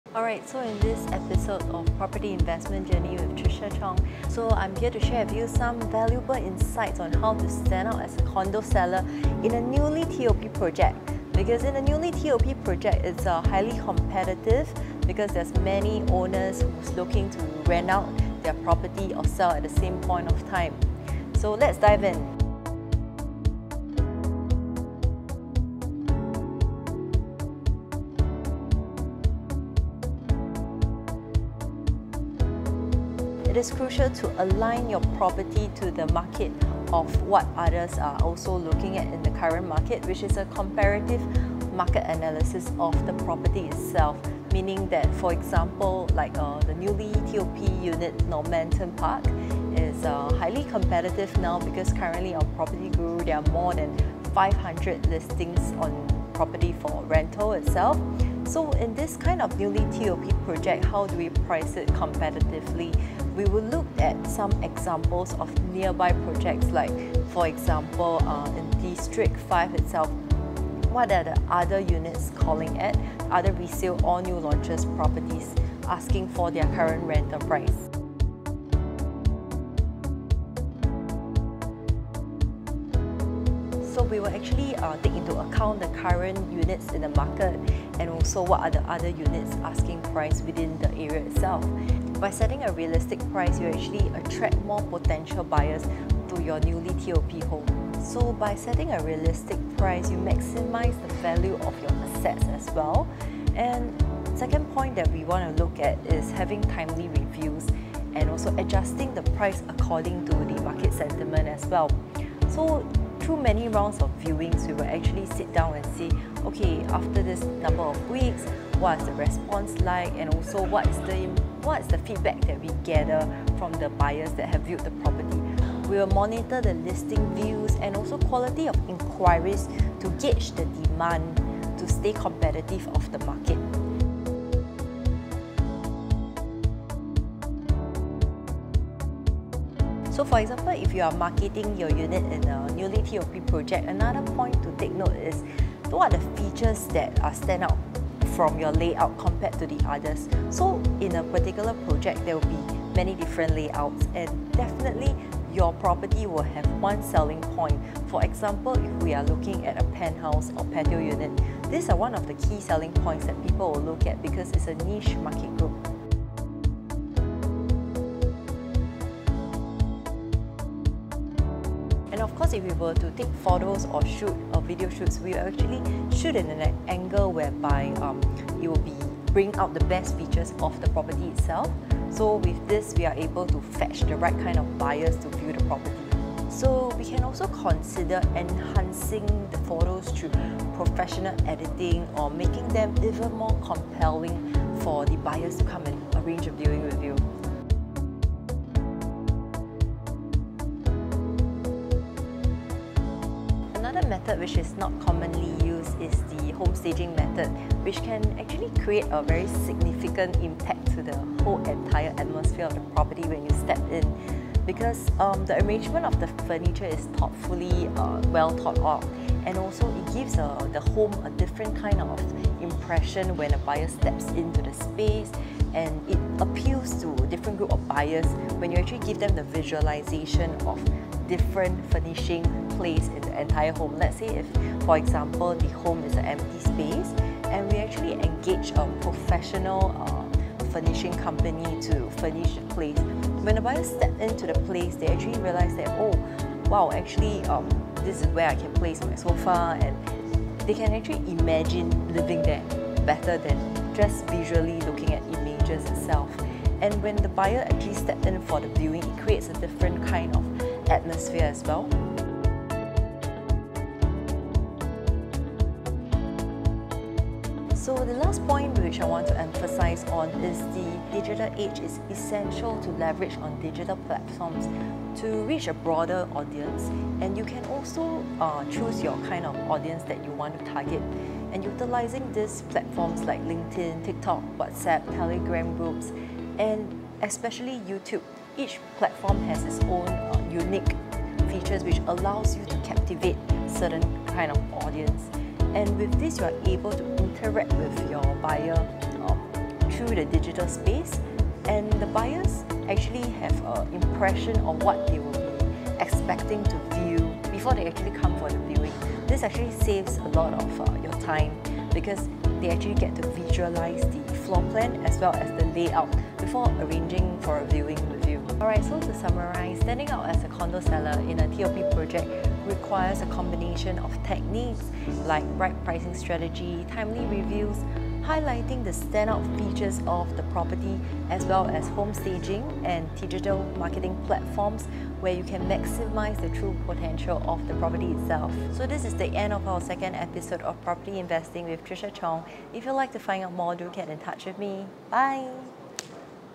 🏙️💎How to stand out as a seller in a Newly TOP project? 🌟
🌟 Ready to Elevate Your Condo Selling Game to New Heights? Join us in this exclusive YouTube video where we unveil the secrets to standing out as a top-notch condo seller!

🏙️💼 From crafting irresistible listings to mastering the art of negotiation, we've got you covered with expert tips, real-life examples, and actionable strategies.

💎 Whether you're a seasoned pro or just starting out, this is your ultimate guide to dominating the condo market and boosting your sales to unprecedented levels!

🚀 Don't miss out – hit play now and revolutionize your approach to condo selling! 💪

To Embark on an Exclusive Journey Through Your Future Home, Feel Free to Get in Touch With:

TRICIA CHONG
R049720J